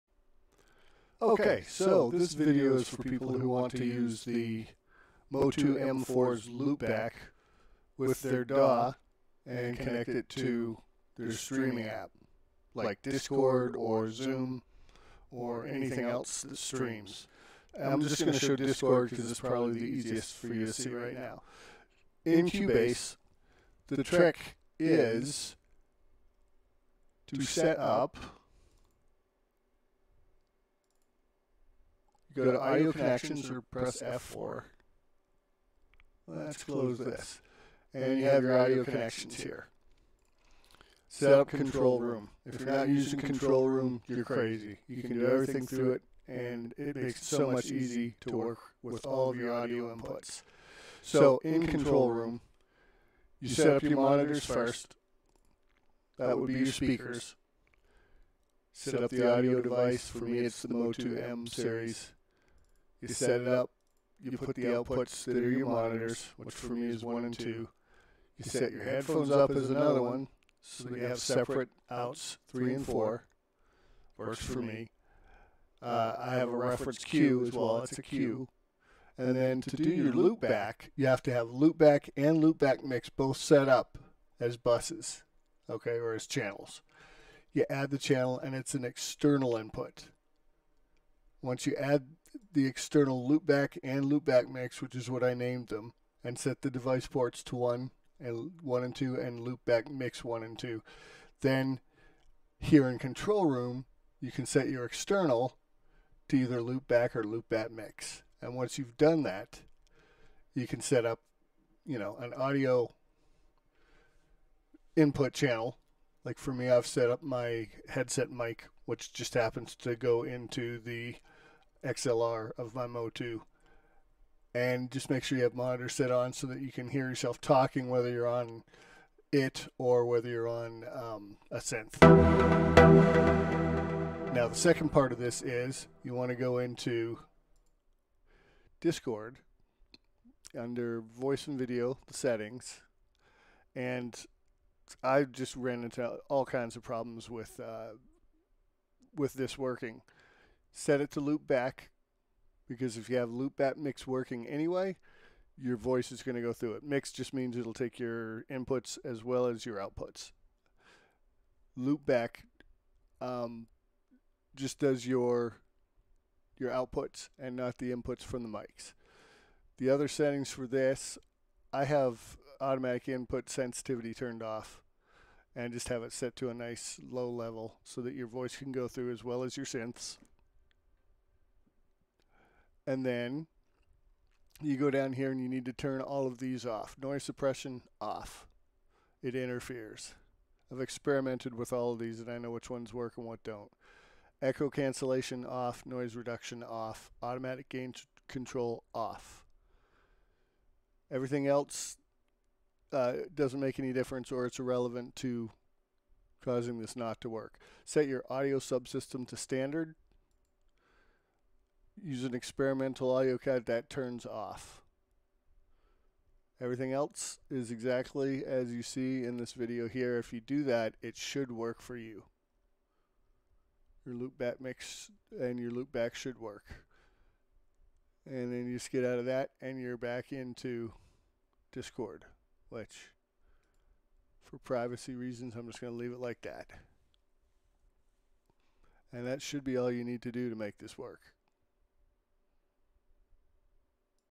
Using MOTU M4 Loopback with Cubase 12 and Discord
How to setup MOTU M4 with Cubase and Discord for streaming. This lets you stream cubase without using windows audio drivers (your MOTU M series ASIO driver doesn't have to be changed) and works properly with the loopback input in discord.